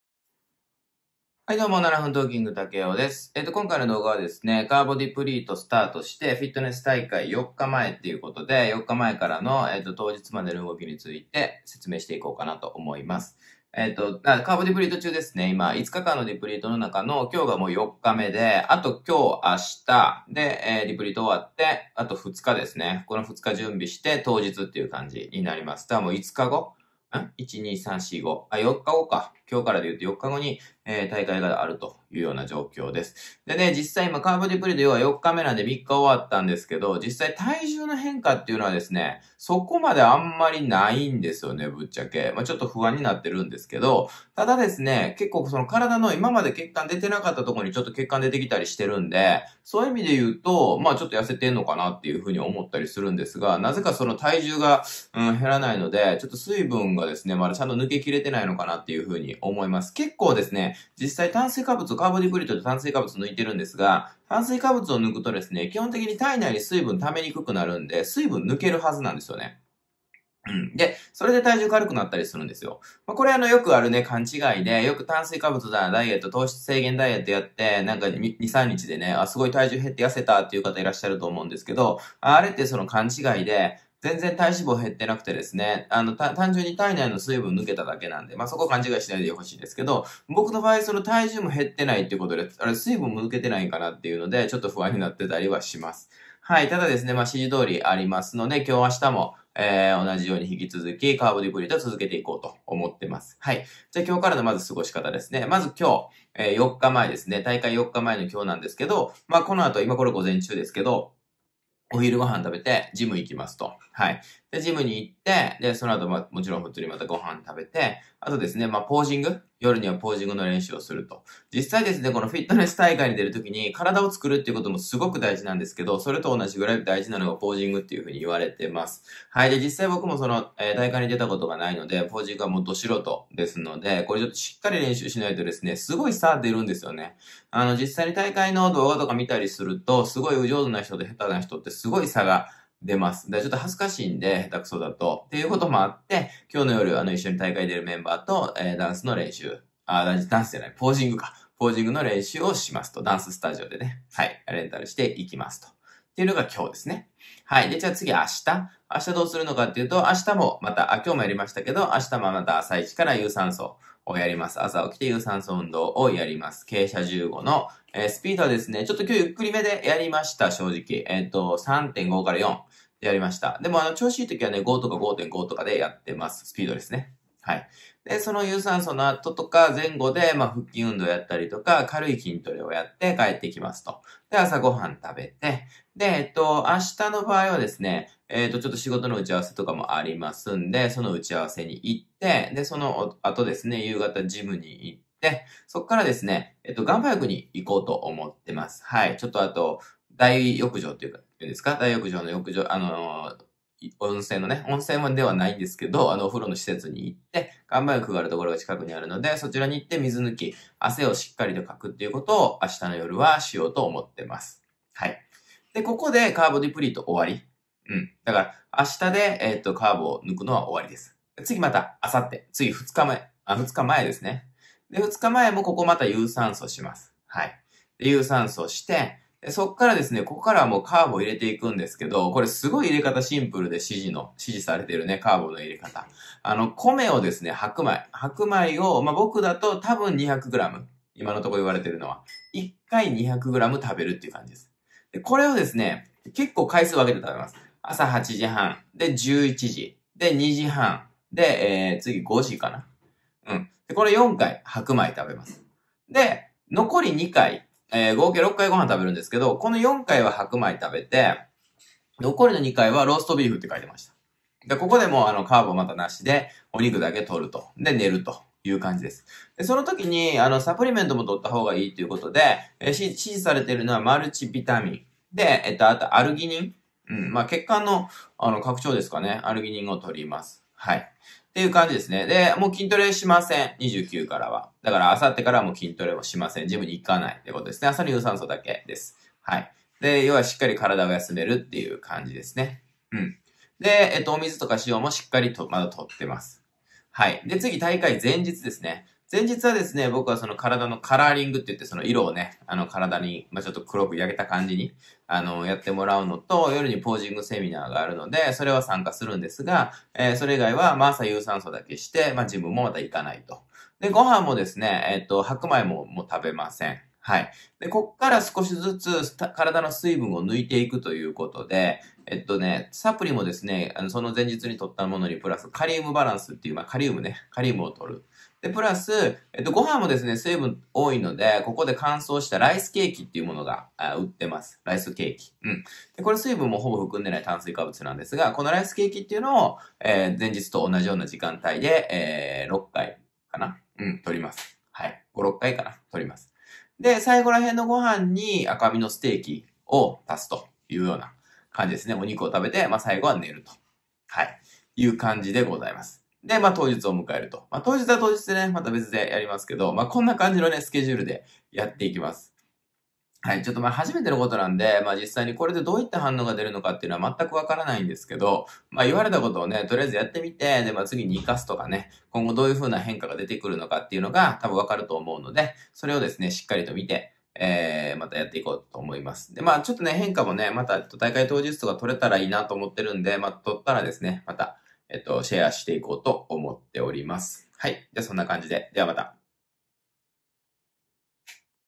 【フィットネス】カーボディプリート4日目、大会4日前。過ごし方。
7分トーキングのTAKEO/タケオです。

フィットネス大会前4日前になりました。カーボディプリートもスタートして4日目になってます。

ここから大会当日までの過ごし方を細かく指示されているのでそちらの方法をシェアしていきます。

※動画編集者、Youtubeチャンネル運営をお手伝いしてくれる方を募集してます。